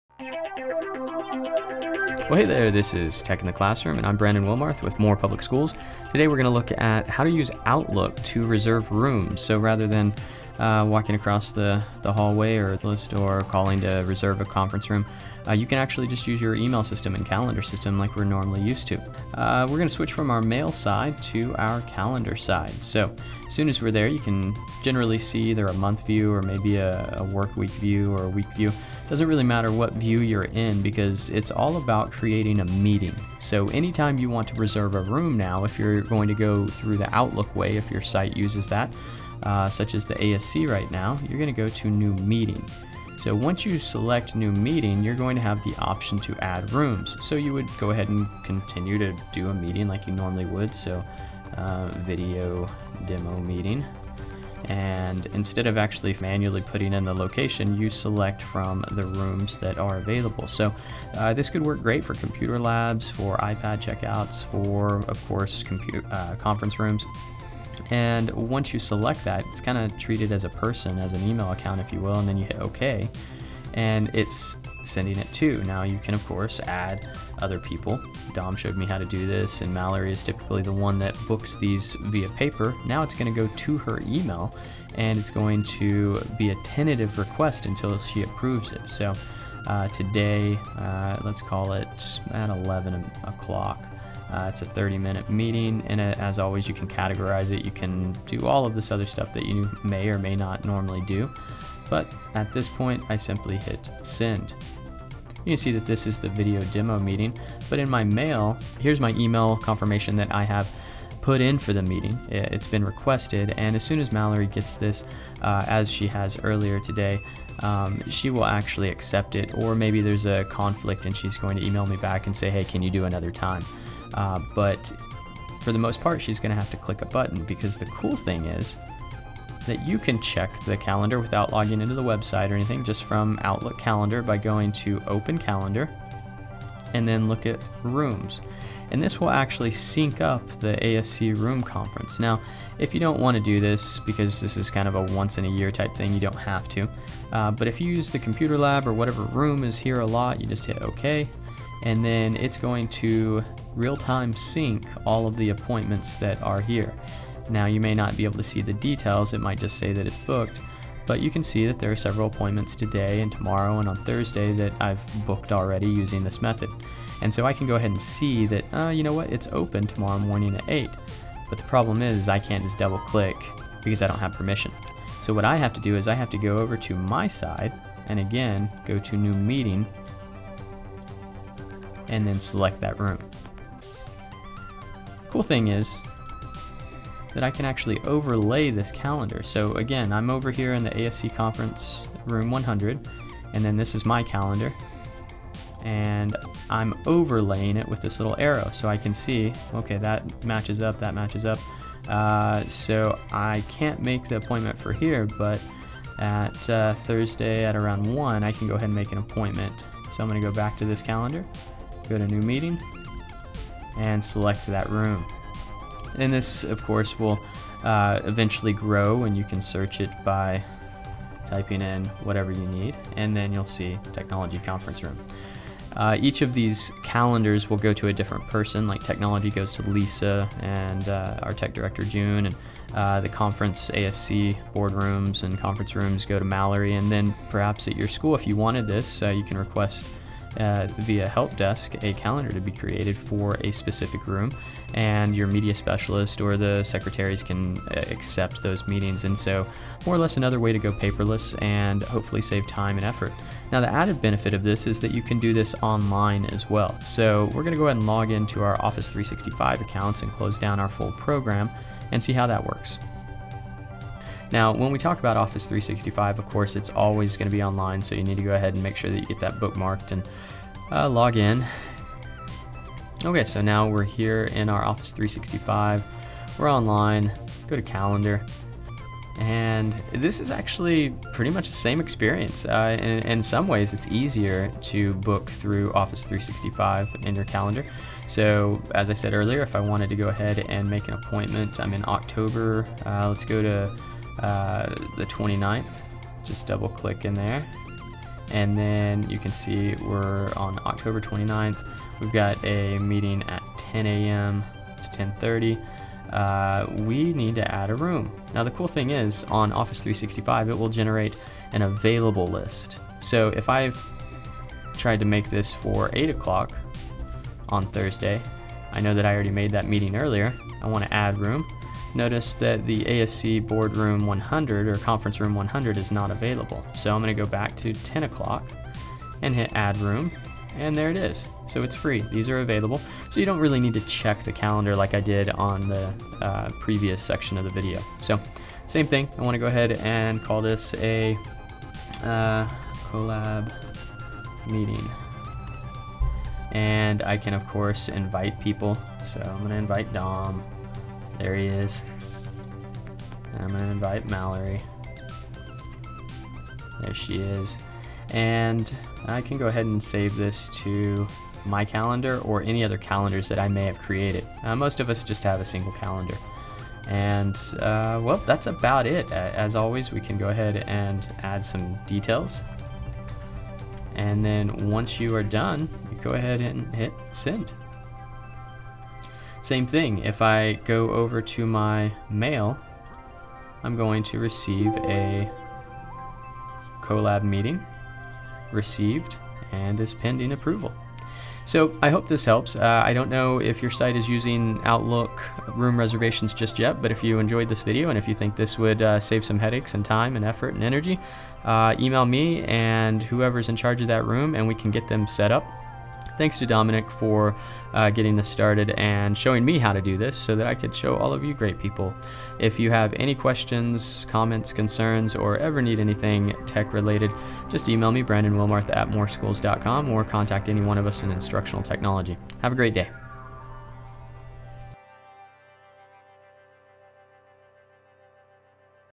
Reserving Rooms with Outlook 2013 & Office 365 Outlook Calendar
Many sites across Moore Schools are trying to cut down on the physical lists, phone calls, and emails that have to happen in order to reserve rooms for student learning and meetings. Here's an in-depth tutorial on how to use Microsoft Outlook and Office 365 to reserve a room right from your calendar.